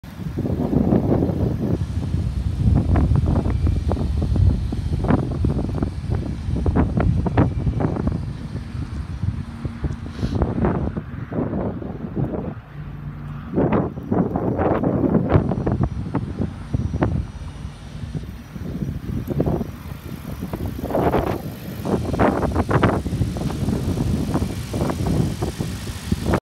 GovDeals: 2014 Ford C-Max Hybrid SE 2.0L, Lic. 10438
2014 Ford C-Max Hybrid SE 2.0L, Lic. 10438 sold by University of Iowa Surplus Vehicle Sales, IA on GovDeals.com